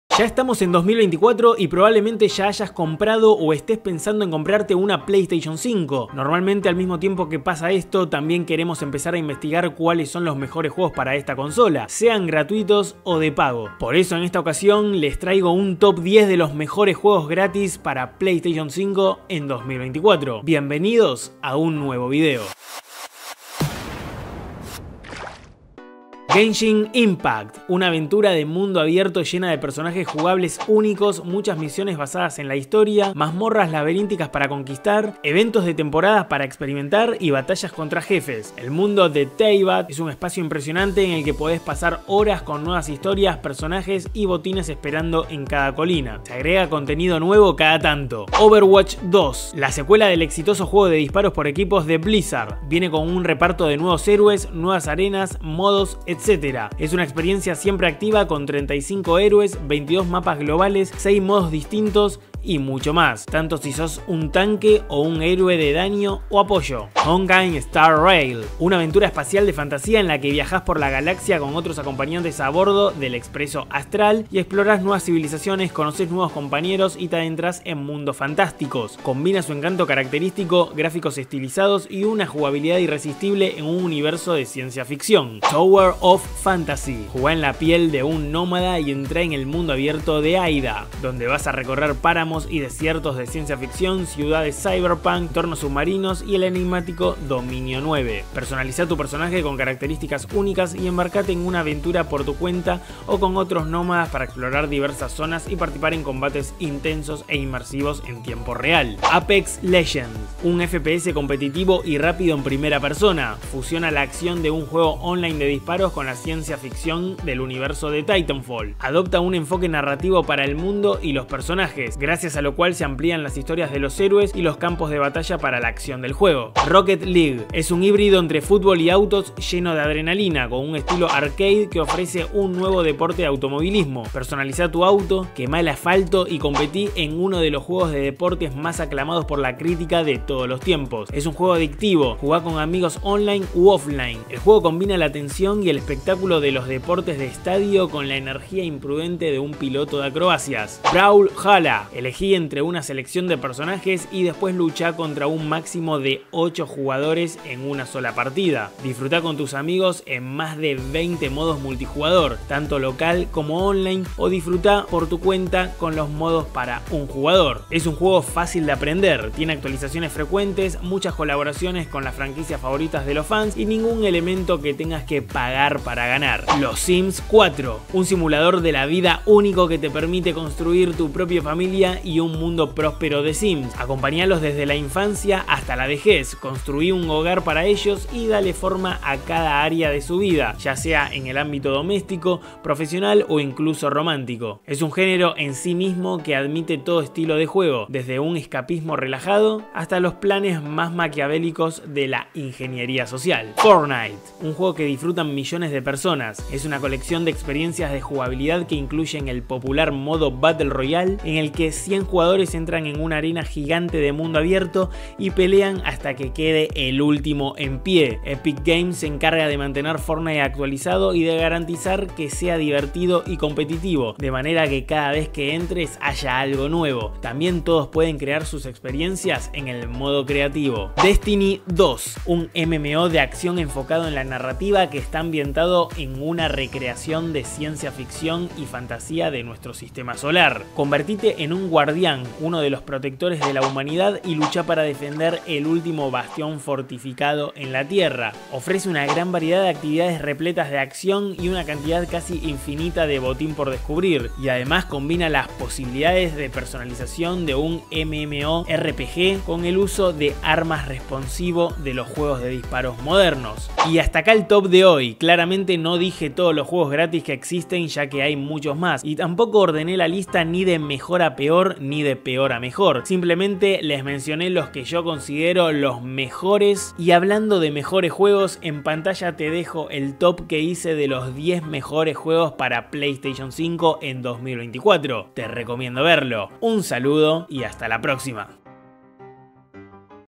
10 MEJORES JUEGOS GRATIS PARA PS5 - 2024
Un TOP 10 de los mejores juegos GRATIS para PlayStation 5 🔥 Si tienes PS5 debes JUGAR ESTOS JUEGOS en 2024!

► ¡SEGUIME!